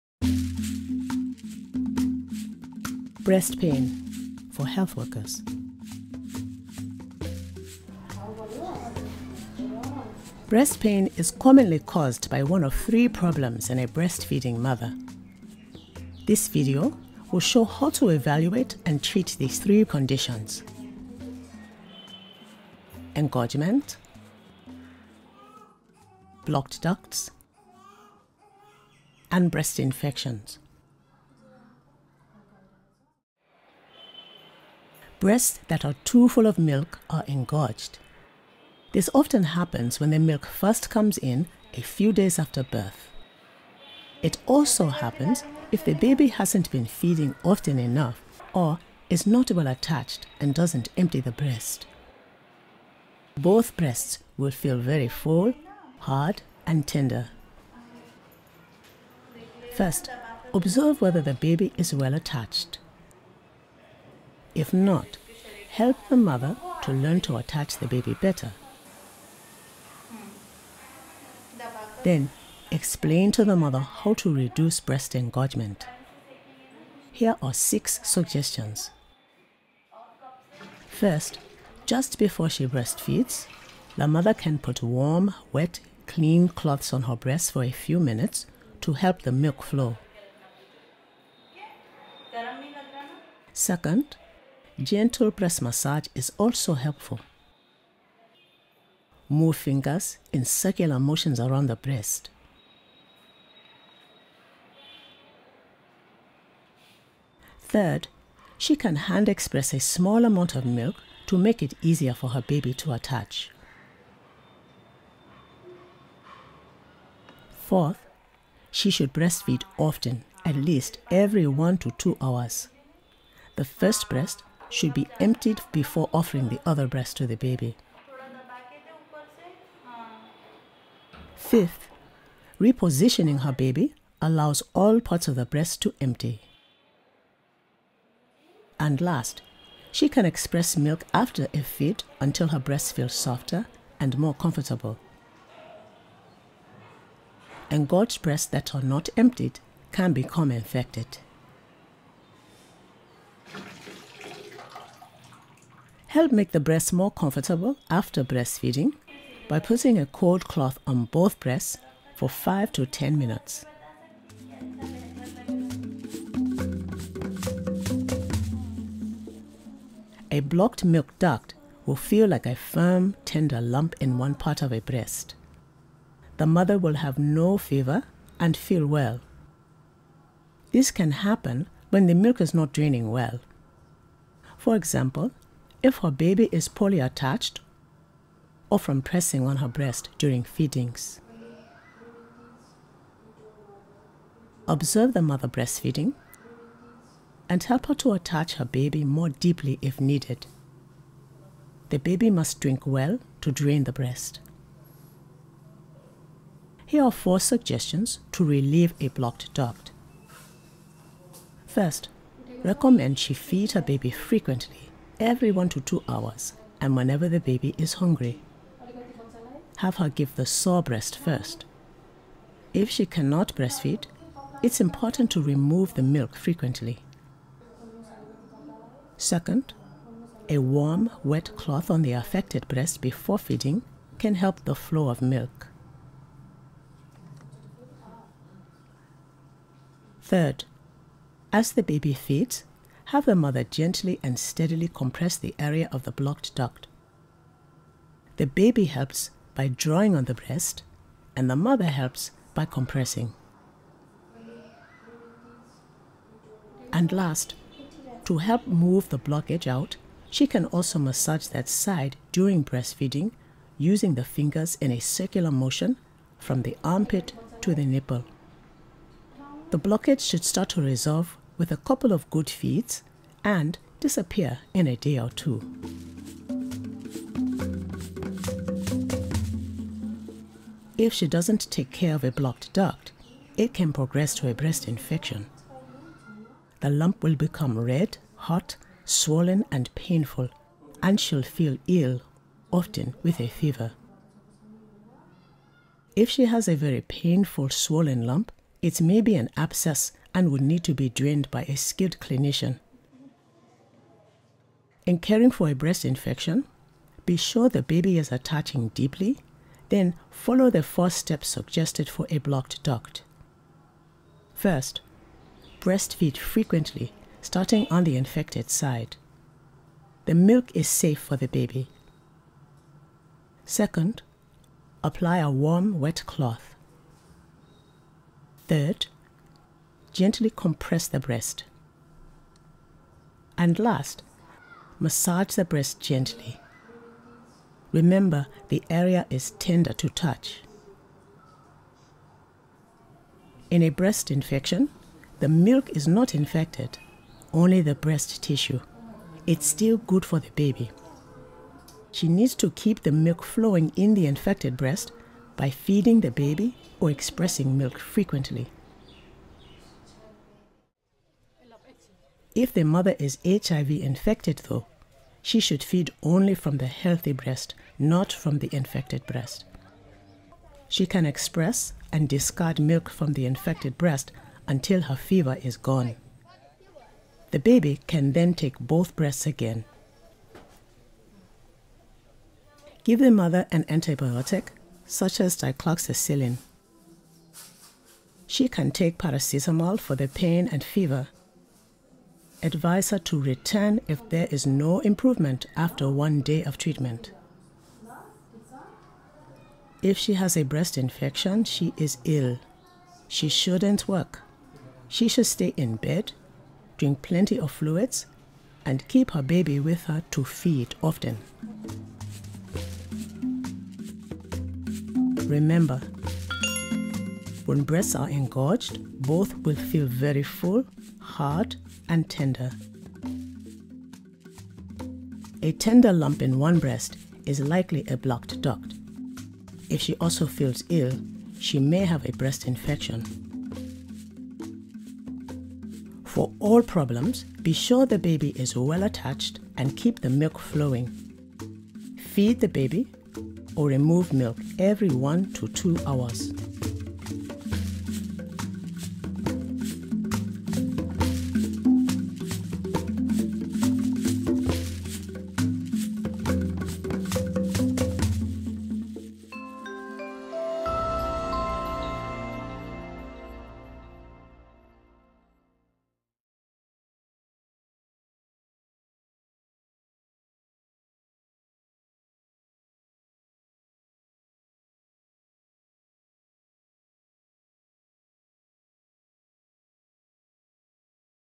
Breast Pain (Chinese subtitles) - Breastfeeding Series
By Global Health Media Project.

Breast pain in a breastfeeding mother is commonly caused by one of three problems: engorgement, blocked ducts, or breast infections. This video shows how to evaluate and treat these 3 conditions.

The intended audience is frontline health workers in the developing world.